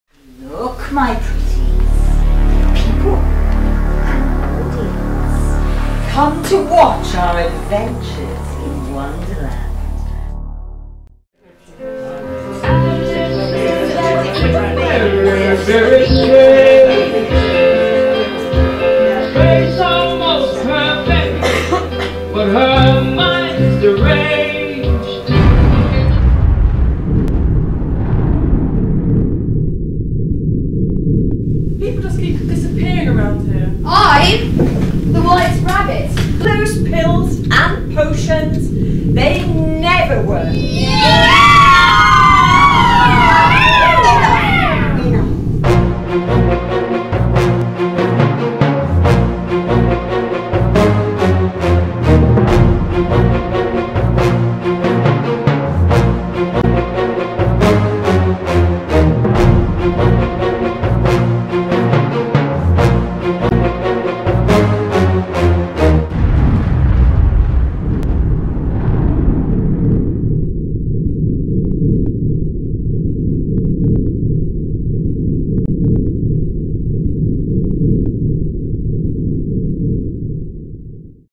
Malice in Wonderland Promo
A Cardington Capers production of Malice in Wonderland by Jim Kirkwood and Sue Tyrrell.